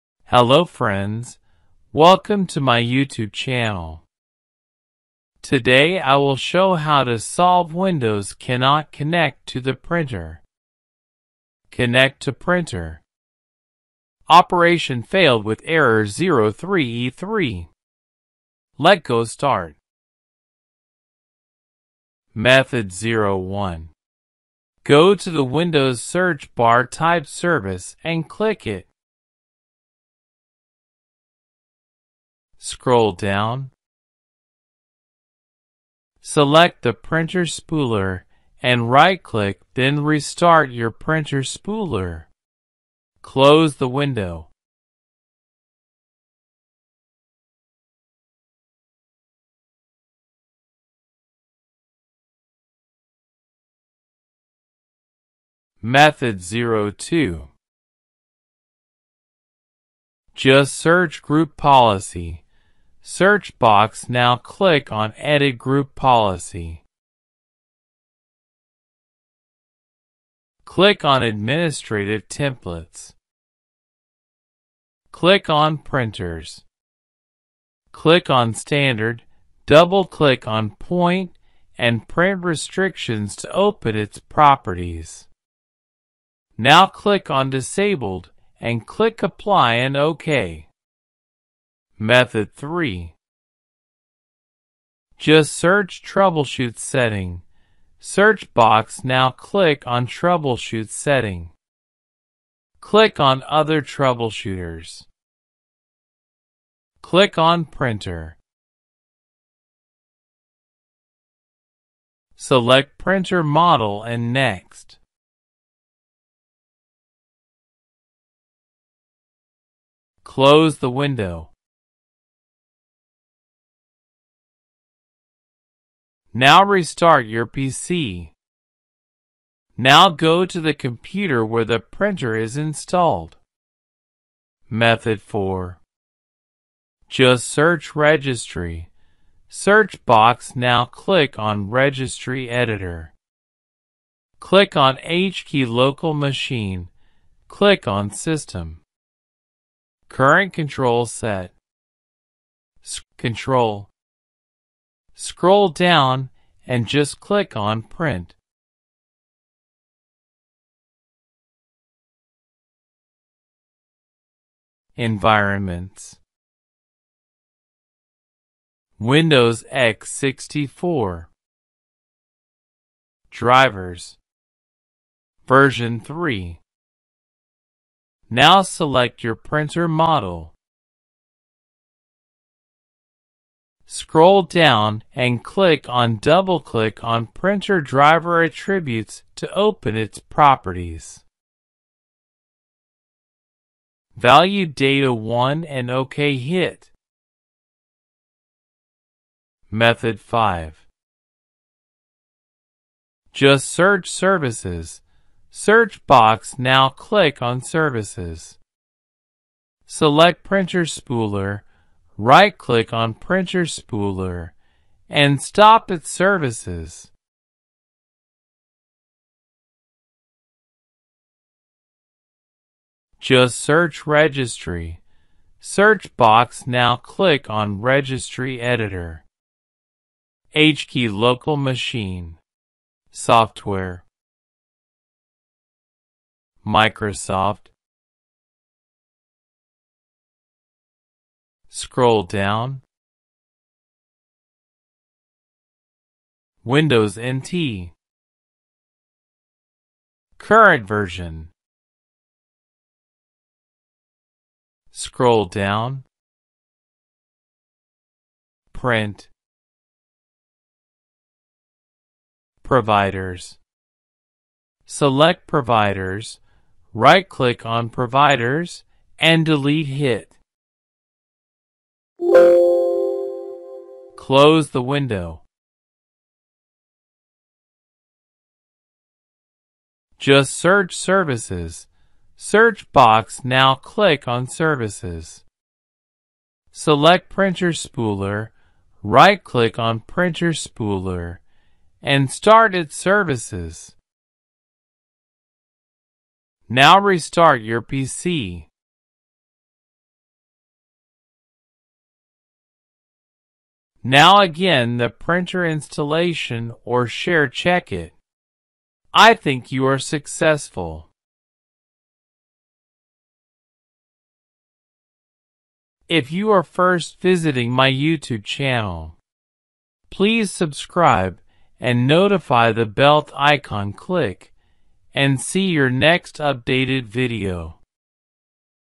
Windows Cannot Connect To The Printer| Operation Failed With Error 0x00003e3
Method-01
Restart the Printer Spooler Service\Service--Printer Spooler

Method-02
Local Group Policy Editor\Administrative templates\Printers\Point and print Restrictions\ select Disabled\apply\Ok

Method-03
Troubleshoot setting\ other Troubleshooters\printer\select printer model\ next\ ok

Method-04
HKEY_LOCAL_MACHINE\SYSTEM\CurrentControlSet\Control\Print\Environments\Windows x64\Drivers\Version-3\select printer model\Printer Driver Attributes\value-1

Method-05
HKEY_LOCAL_MACHINE\SOFTWARE\Microsoft\Windows NT\CurrentVersion\Print\providers

Windows 11
Windows 10